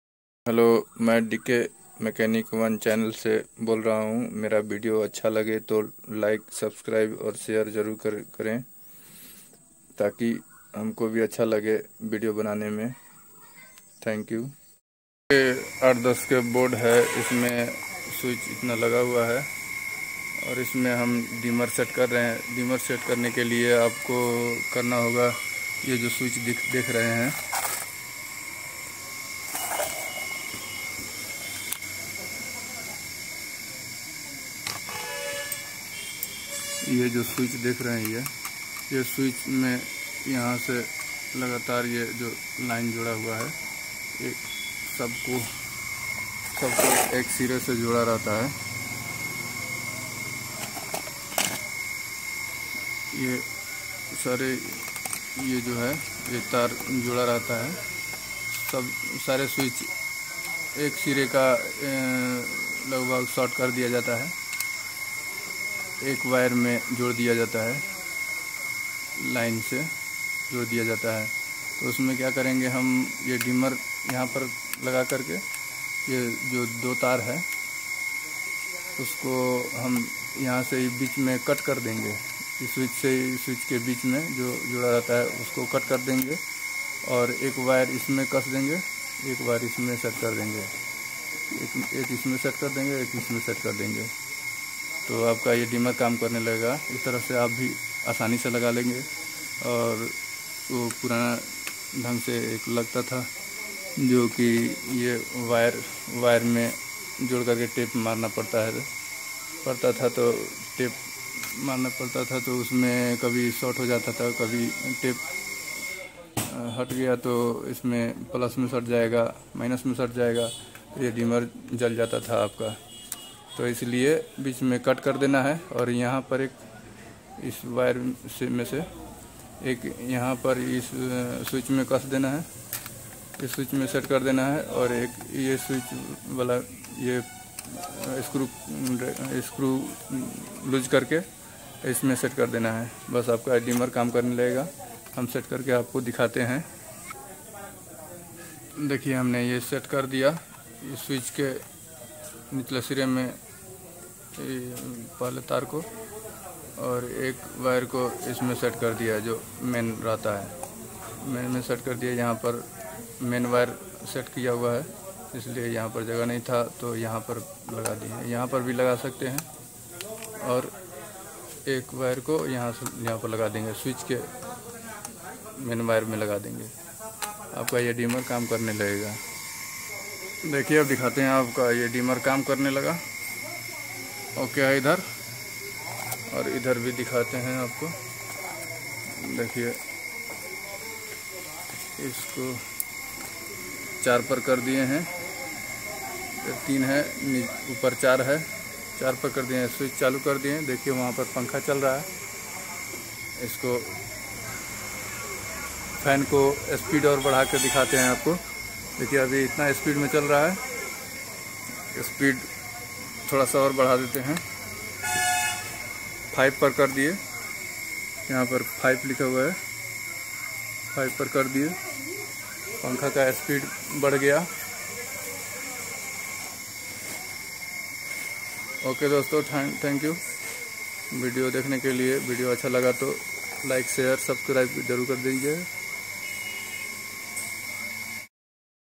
Install dimmer in room board in this way in Hindi //रूम के बोर्ड में डिमर इस तरह से लगाएं //
Dk mechanic1 channel me swagat hai //
deemer lagane ka aasan tarika // room ke boards me deemer kaise lagayajata hai sekhe es video me //
रूम के बोर्ड में डिमर कैसे लगाया जाता हैं देखें विडियो में // हिंदी में //